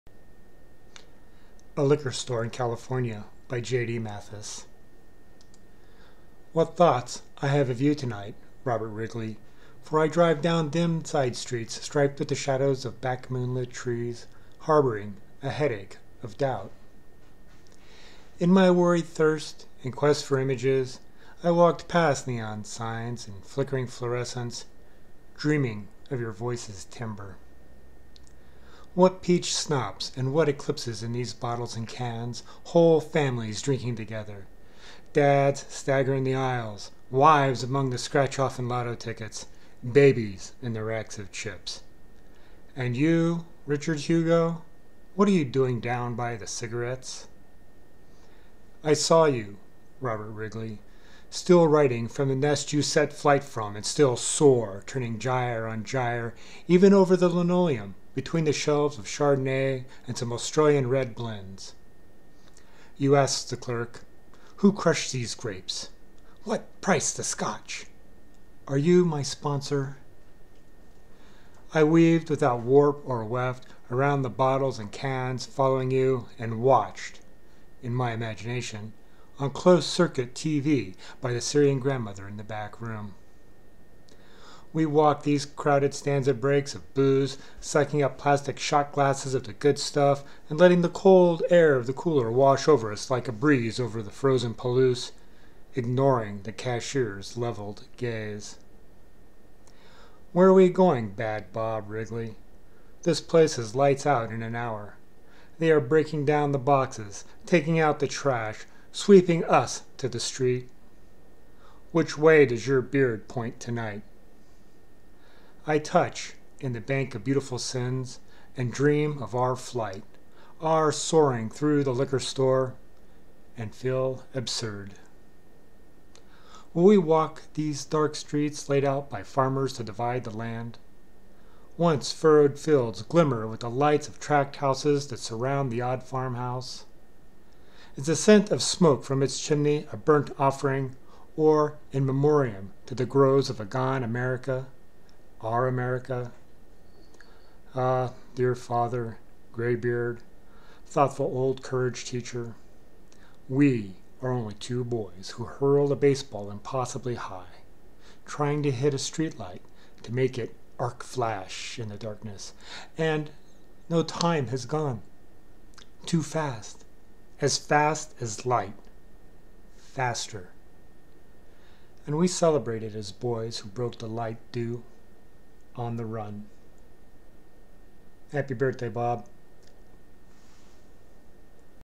A Liquor Store in California
A birthday poem for Robert Wrigley based on Allen Ginsberg's "A Supermarket in California."
*Read it Below*
By J.D. Mathes

     What thoughts I have of you tonight, Robert Wrigley, for I drive down dim side streets striped with the shadows of back-moon-lit trees, harboring a headache of doubt.
 In my weary thirst, and quest for images, I walked past neon signs and flickering fluorescents, dreaming of your voice’s timbre.

     What peach schnapps and what eclipses in these bottles and cans! Whole families drinking together! Dads stagger in the aisles! Wives among the scratch-off and lotto tickets, babies in the racks of chips! And you, Richard Hugo, what were you doing down by the cigarettes?

I saw you, Robert Wrigley, still writing from the nest you set flight from and still soar, turning gyre on gyre, even over the linoleum between the shelves of chardonnay and some Australian red blends. You ask the clerk: Who crushed these grapes? What price the scotch?

     I weaved without warp or weft around the bottles and cans following you and watched, in my imagination, on close circuit tv by the Syrian grandmother in the backroom.

    We walk these crowded stanza breaks of booze, sucking up plastic shot glasses of the good stuff and letting the cold air of the cooler wash over us like a breeze over the frozen Palouse, ignoring the cashier’s leveled gaze.

     Where are we going, Bad Bob Wrigley? This place is lights-out in an hour. They are breaking down the boxes, taking out the trash, sweeping us to the street. Which way does your beard point tonight?

 (I touch In the Bank of Beautiful Sins and dream of our flight, our soaring through the liquor store and feel absurd.)

     Will we walk these dark streets laid out by farmers to divide the land? Once furrowed fields glimmer with the lights of tract houses that surround the odd farmhouse. Is the scent of smoke from its chimney a burnt offering or in memoriam to the groves of a gone America? Our America.

     Ah, dear father, graybeard, thoughtful old courage teacher. We are only two boys who hurl a baseball impossibly high, trying to hit a streetlight to make it “arc-flash” in the darkness and know time has gone “too fast, as fast as light, faster” and celebrate it as boys who broke the light do, on the run.

Tehachapi, 2021

Material in quotes from "Speed of Light," In the Bank of Beautiful Sins by Robert Wrigley.